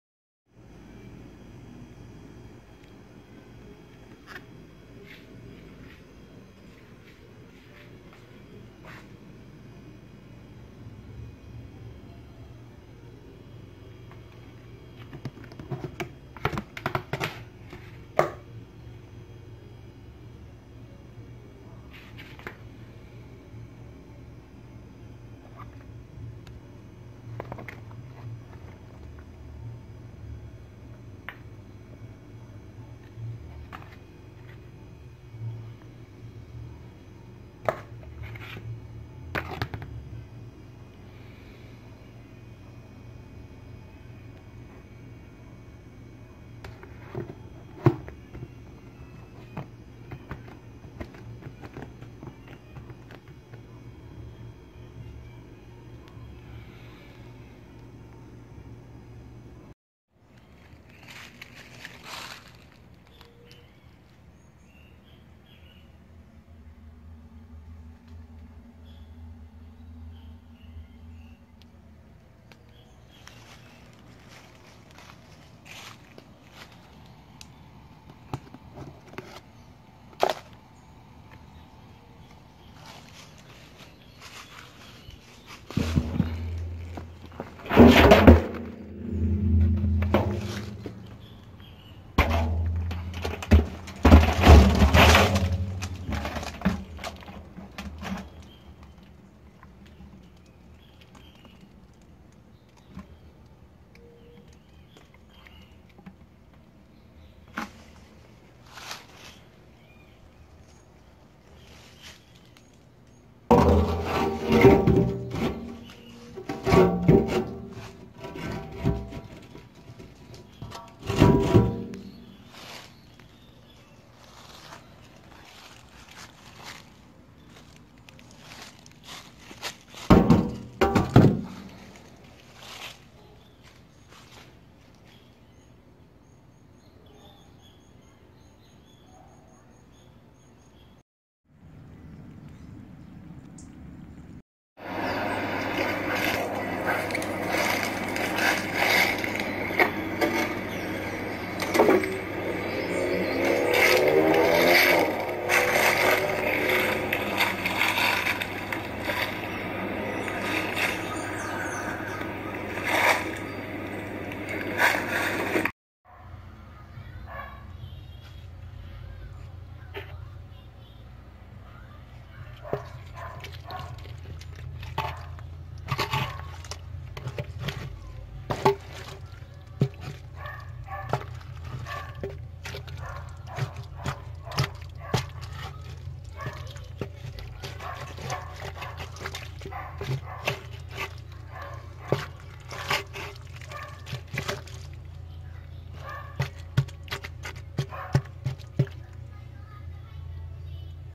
Anaerobic Composting Without Drainage Holes
What happens when you compost in a barrel anaerobically without drainage holes